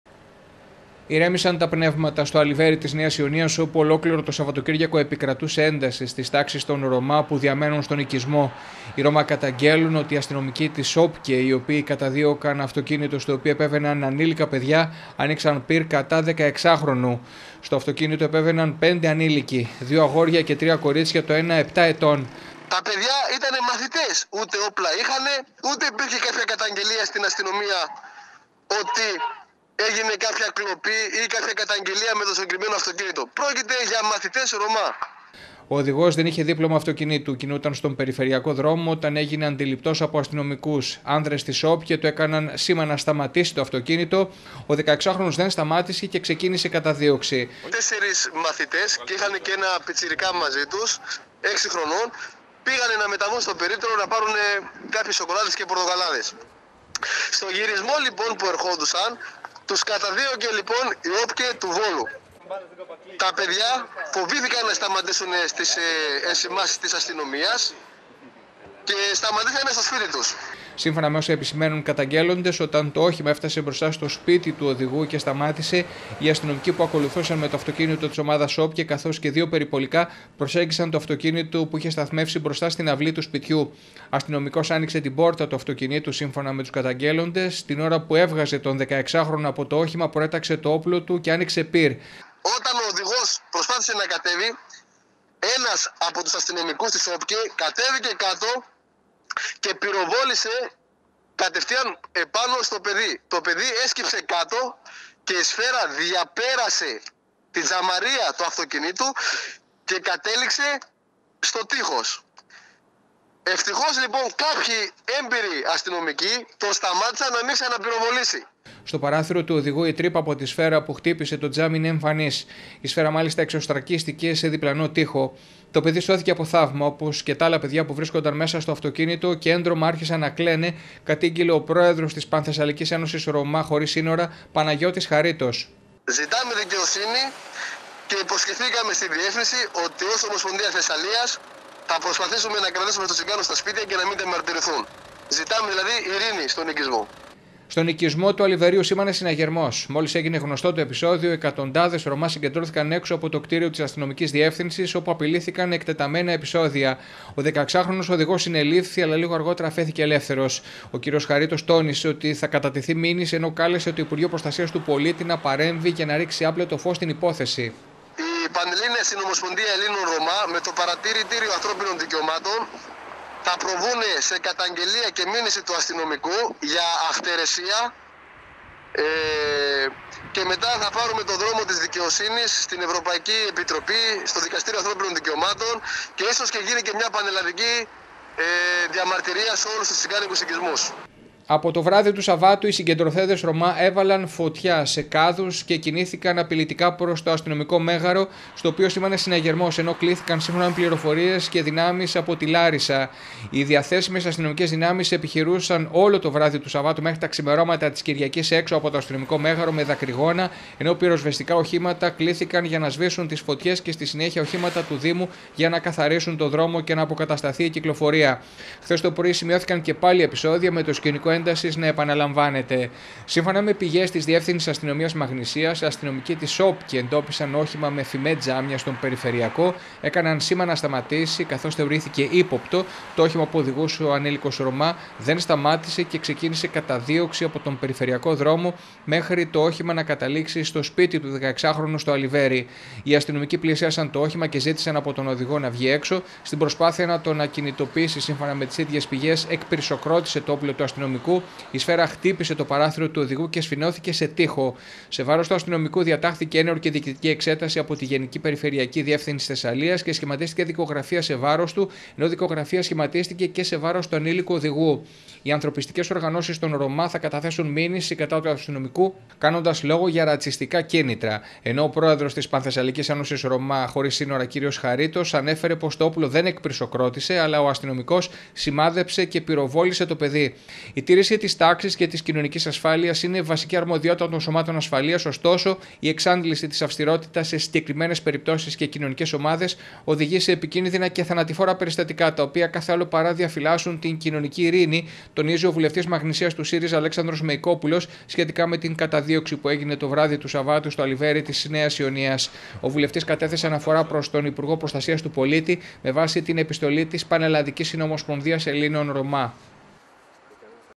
Βόλος Στο πόδι ο οικισμός Αλιβερίου Καταγγέλλεται πυροβολισμός αστυνομικού κατά 16χρονου Ρομά 31012
Αναστατωμένη παραμένει η κοινότητα των Ρομά στο Αλιβέρι της Νέας Ιωνίας, μετά το επεισόδιο σε βάρος ανηλίκων Ρομά, το Σάββατο το βράδυ. Για φωτιές σε κάδους απορριμμάτων, αλλά και στο ρέμα που διαπερνά τον οικισμό, κλήθηκαν δυνάμεις της Πυροσβεστικής Υπηρεσίας. Όλα ξεκίνησαν όταν αστυνομικοί της ομάδας ΟΠΚΕ άρχισαν να καταδιώκουν στον περιφερειακό δρόμο αυτοκίνητο, ο οδηγός του οποίου δεν σταμάτησε σε σήμα τους. Η καταδίωξη ολοκληρώθηκε μόλις ο οδηγός, ένας 16χρονος ρομά, έφτασε στο σπίτι του. Εκεί - όπως καταγγέλλεται - αστυνομικός έβγαλε το όπλο του και πυροβόλησε, χωρίς ευτυχώς η σφαίρα να τραυματίσει τον ανήλικο οδηγό, ή κάποιον άλλον από του πέντε ανήλικους συνεπιβαίνοντες. Το όπλο εκπυρσοκρότησε, την ώρα που ο αστυνόμος προσπαθούσε να το βάλει στη θήκη του” είπε ο αστυνομικός διευθυντής Μαγνησίας. Εναντίον του αστυνομικού σχηματίσθηκε δικογραφία και διατάχθηκε ΕΔΕ. Οι ανθρωπιστικές οργανώσεις των ΡΟΜΑ θα καταθέσουν μήνυση κατά του αστυνομικού, κάνοντας λόγο για ρατσιστικά κίνητρα.